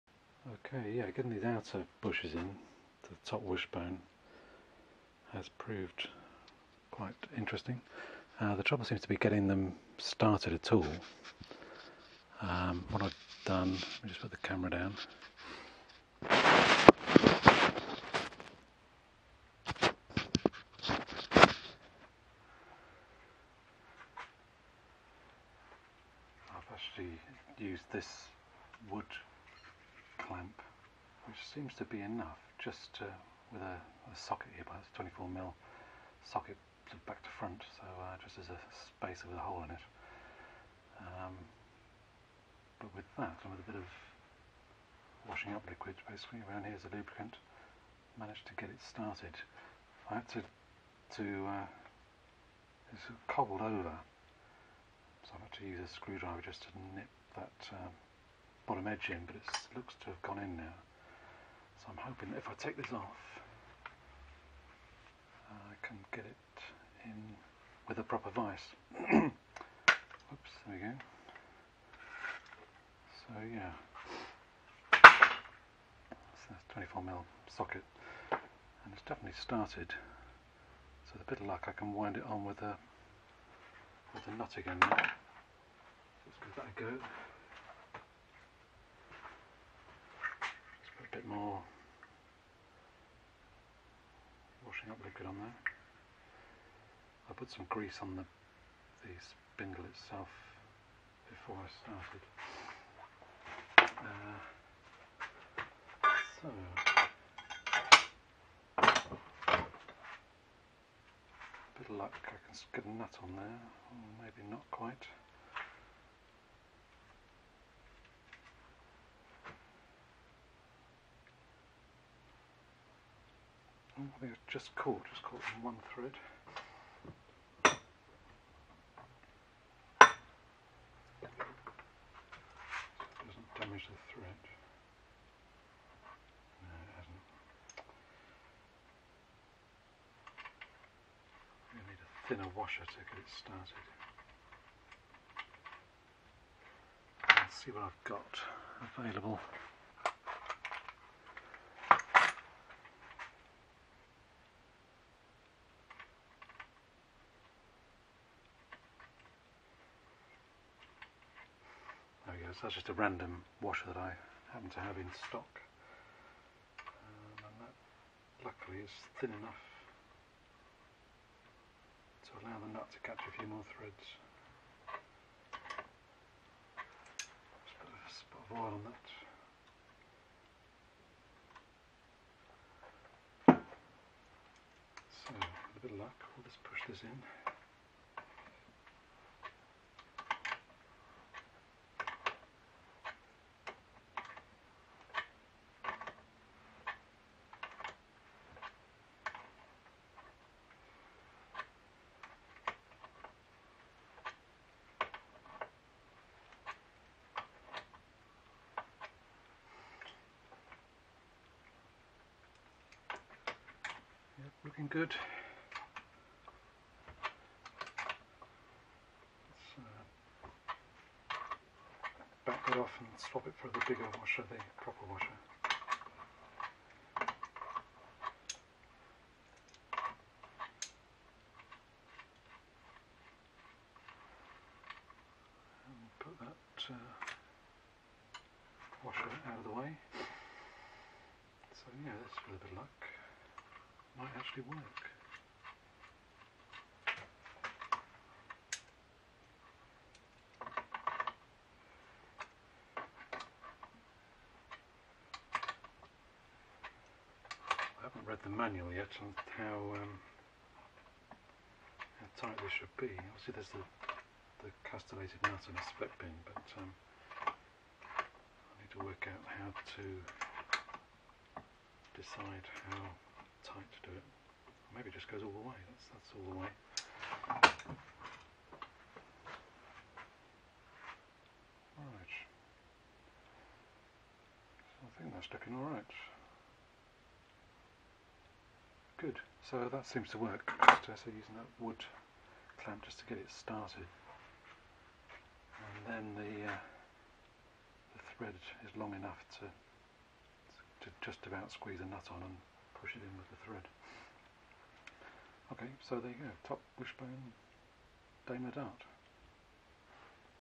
Daimler SP250 Dart Top Wishbone Outer Bush Fitment
My attempt to fit the outer PU bush to my Daimler dart top wishbone
[L]